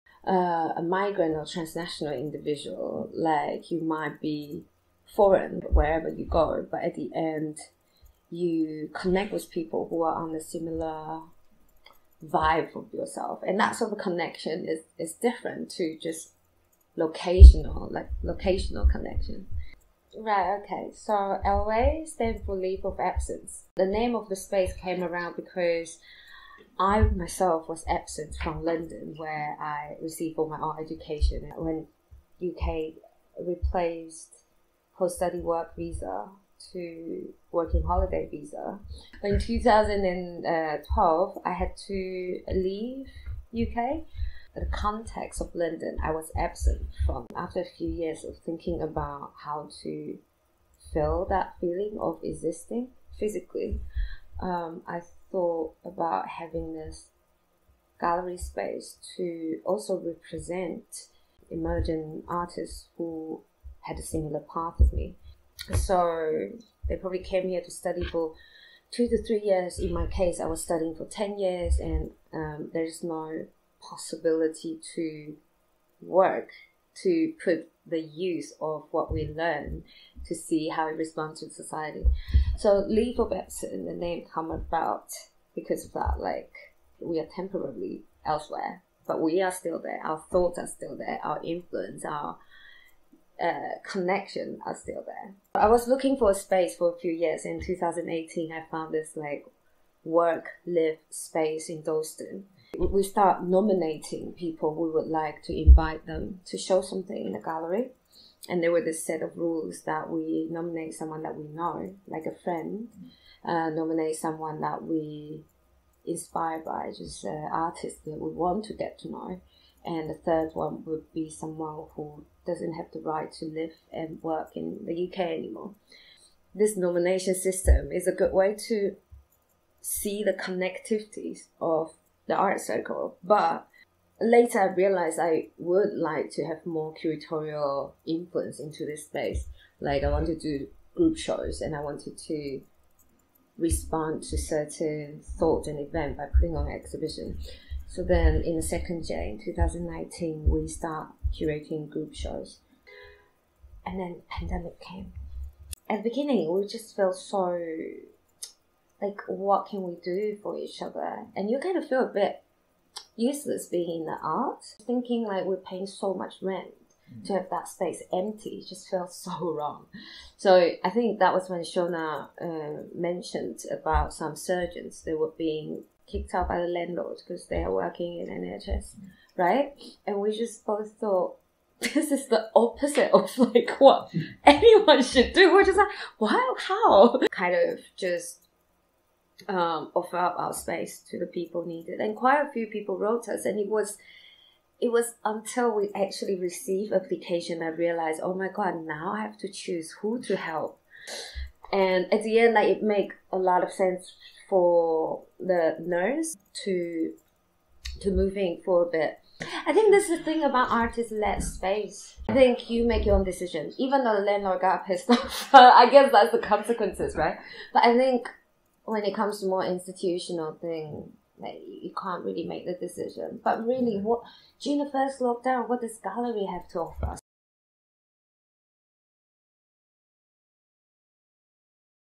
Sluice encounters | Leave Of Absence
Sluice popped over to the Delfina Foundation where Yu'an Huang is currently undertaking a residency to talk about Leave Of Absence – a curator-run gallery founded by Yu'an in Dalston in 2018. Yu'an talks about how the transnational migrant experience forms the backbone of LOA and what happened when the pandemic hit.

For an in-depth interview with Yu'an pick up the latest edition of Sluice magazine, available here: